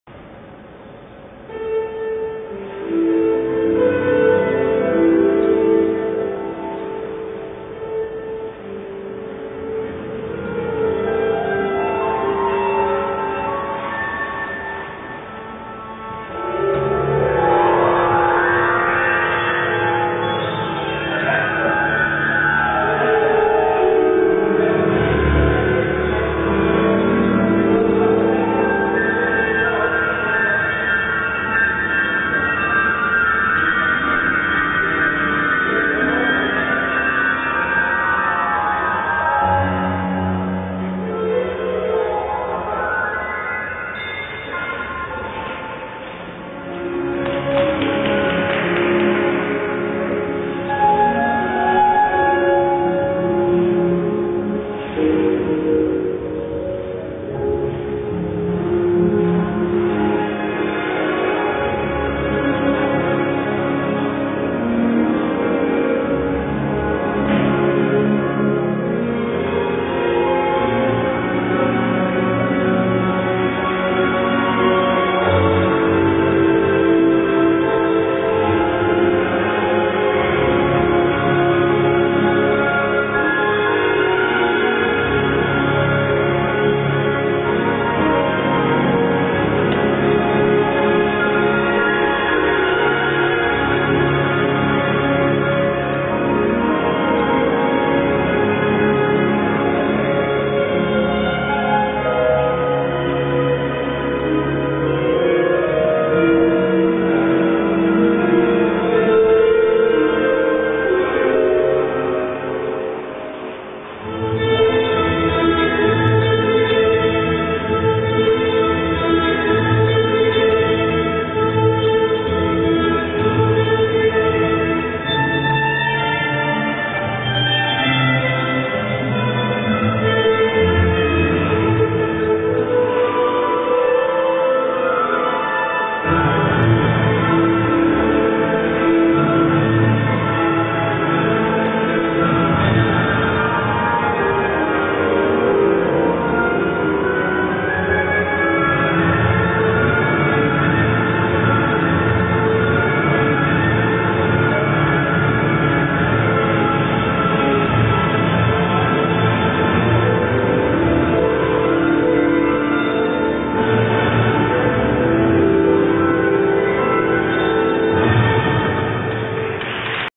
3 Promsan RATTANADILOK NA PHUKET (THA) - ISU JGP Austria 2012 Junior Ladies Short Program
To license ISU footage 3 Promsan RATTANADILOK NA PHUKET (THA) - ISU JGP Austria 2012 Junior Ladies Short Program